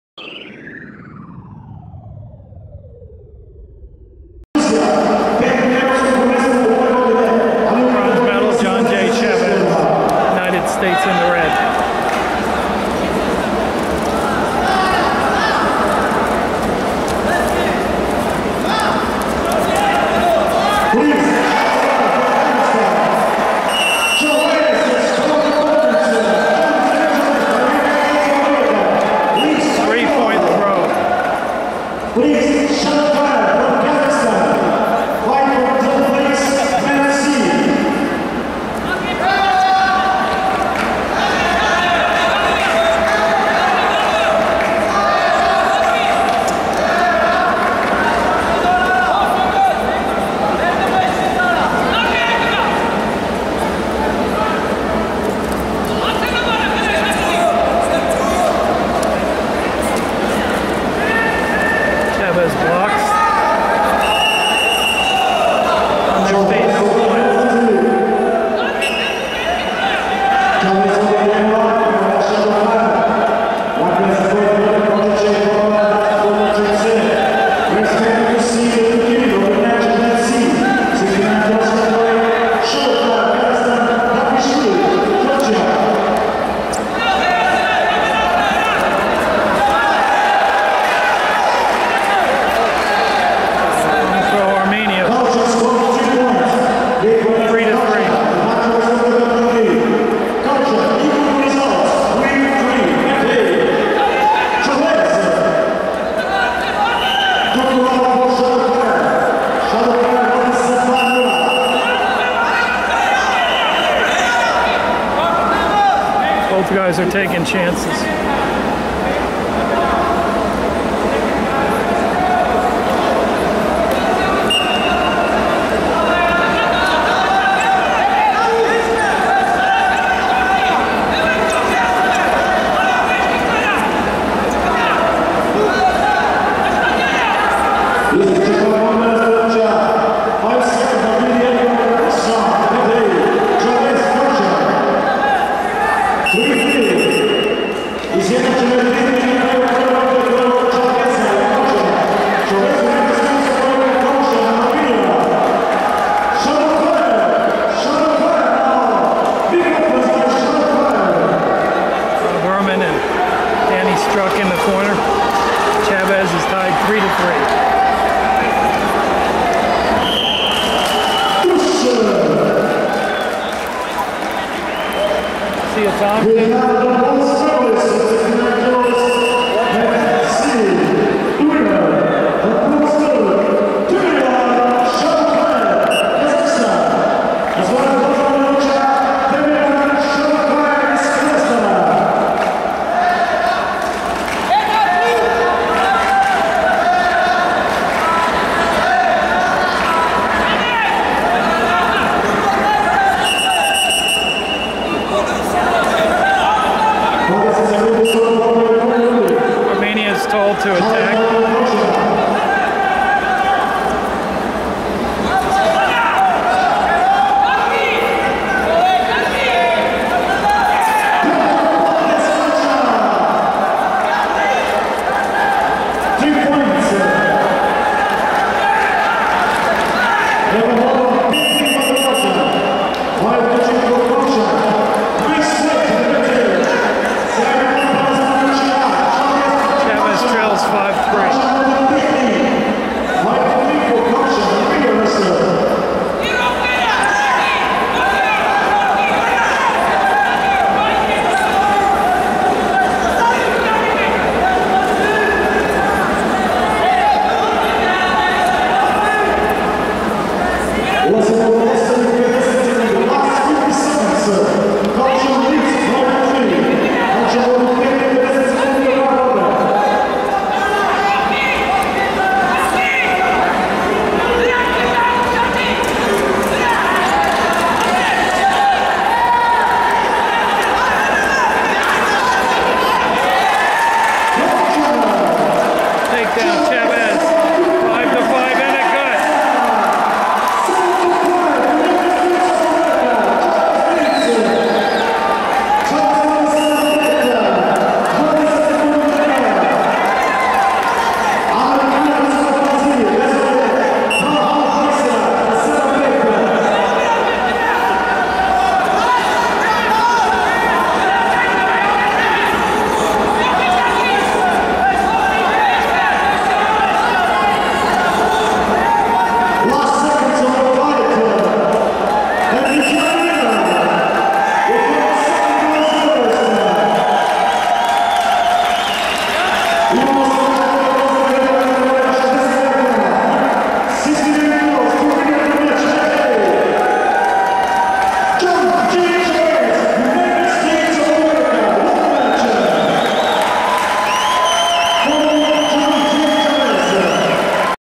Cadet W GR - Chavez (USA) dec. Kalchyan (ARM), 69 kg bronze medal bout
Jon Jay Chavez (USA) dec. Hrant Kalchyan (Armenia), 8-5 in bronze medal bout at 69 kg in Greco-Roman at Cadet World Championships in Zrenjanin, Serbia